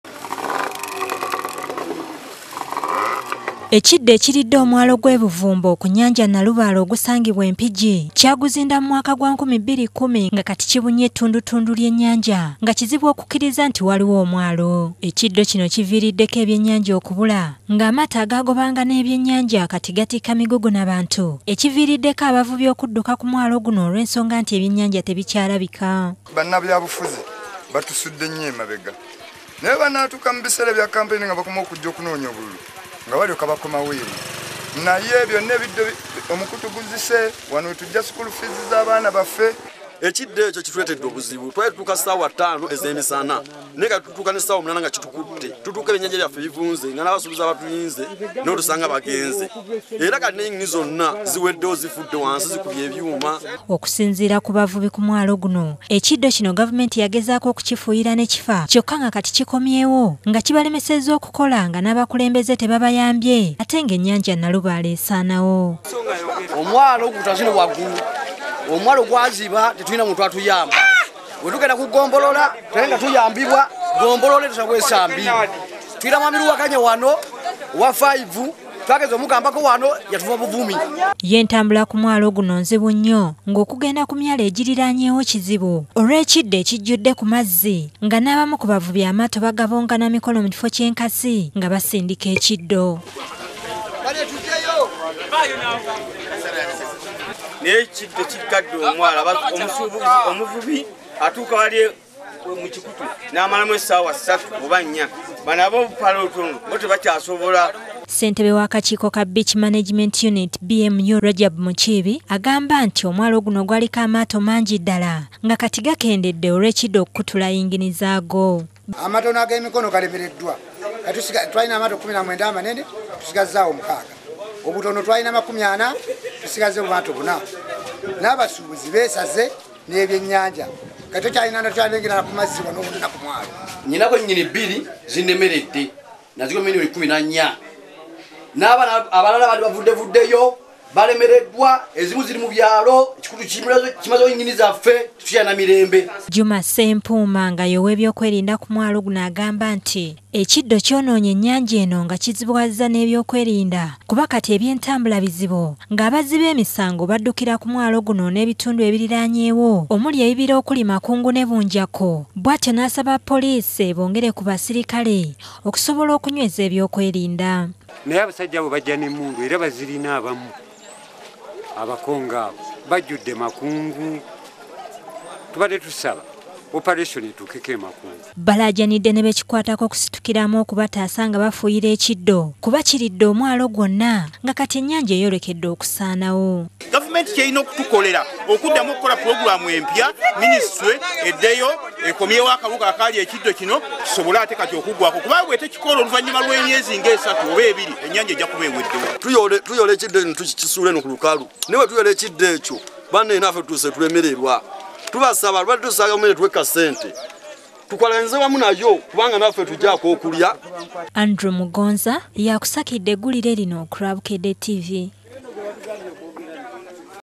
Ekiddo kimazeewo omwalo e Kammengo
Abavubi balaajana lwa yingini kukuba